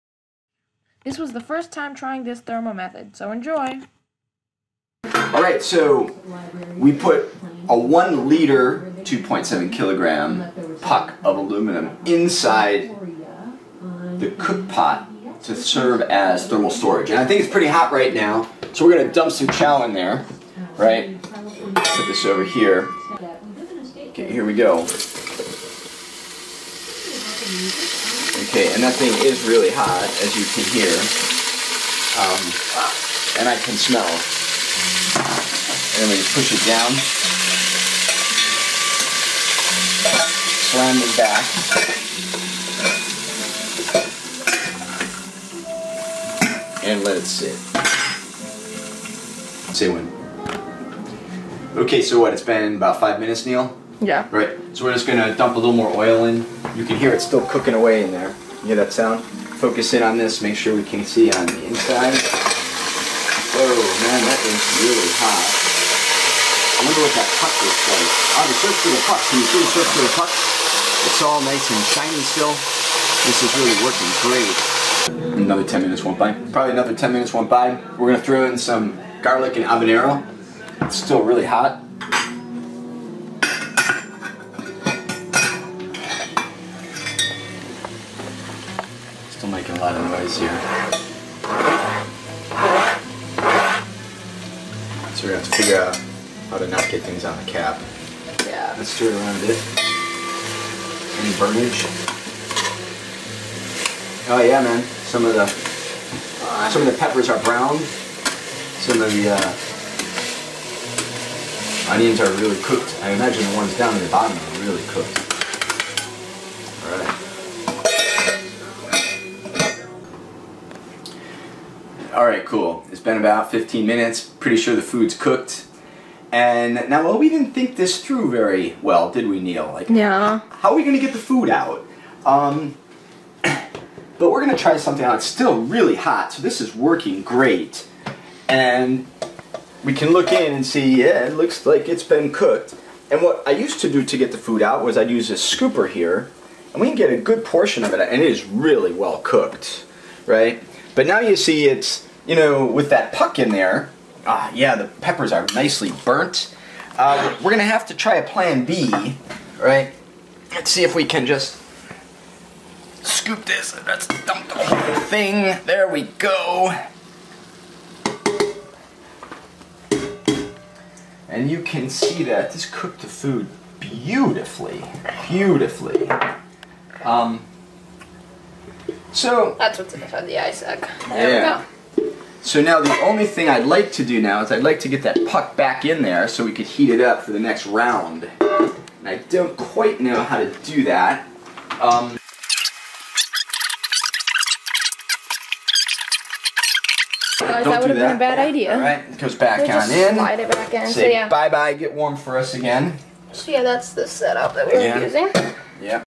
ISEC cooking, Aluminum Puck Thermal Storage, Pete Schwartz, Cal Poly Physics
We stored two hours of thermal energy from a 100 W solar electric heater in a 2.7 kg aluminum puck - located directly in the cookpot.  This is the first time we tried it, and we were surprised at how well it worked.  It was a little more difficult getting the food out of the cookpot.  Filmed and produced by Neil Schwartz